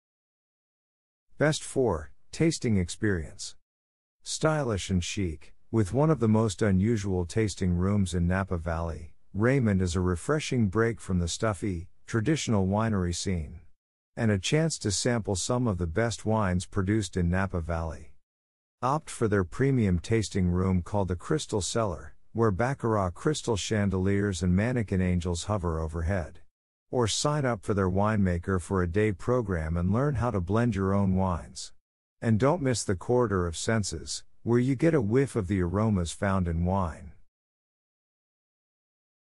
Raymond Vineyards
Best for: Tasting experience.
Stylish and chic, with one of the most unusual tasting rooms in Napa Valley, Raymond is a refreshing break from the stuffy, traditional winery scene. And a chance to sample some of the best wines produced in Napa Valley..
Opt for their premium tasting room called the Crystal Cellar, where Baccarat crystal chandeliers and mannequin angels hover overhead. Or sign up for their winemaker for a day program and learn how to blend your own wines. And don't miss the Corridor of Senses, where you get a whiff of the aromas found in wine..